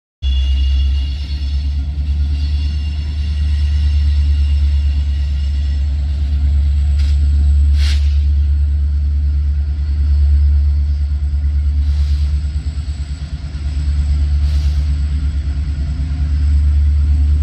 EX BN SD60M 1452 LEADS A EASTBOUND LOCAL THROUGH SANTA FE JUNCTION!
Brand new video from KC with rare units & nearly constant meets a Santa Fe Junction!!!